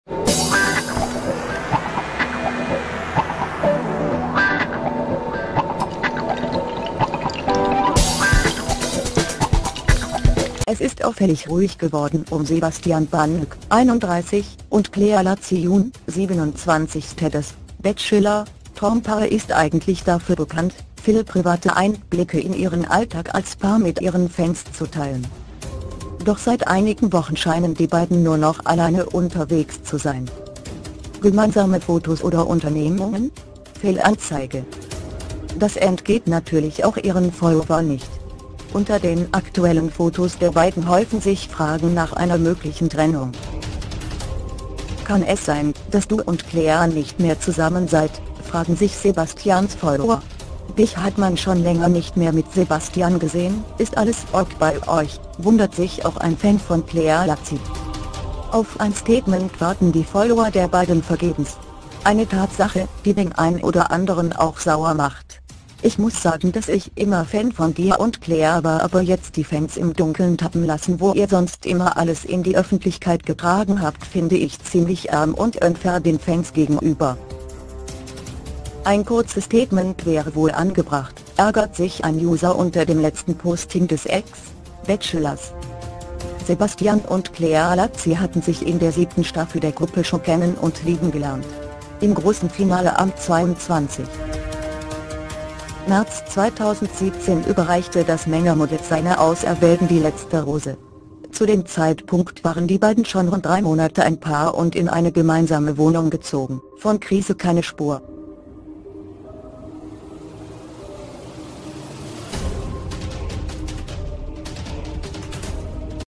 SEBASTIAN PANNEK + CLEA-LACY HEIMLICHE TRENNUNG?
Es ist auffällig ruhig geworden um Sebastian Pannek, 31, und Clea-Lacy Juhn, 27. Das "Bachelor"-Traumpaar ist eigentlich dafür bekannt, viele private Einblicke in ihren Alltag als Paar mit ihren Fans zu teilen.
 Doch seit einigen Wochen scheinen die beiden nur noch alleine unterwegs zu sein.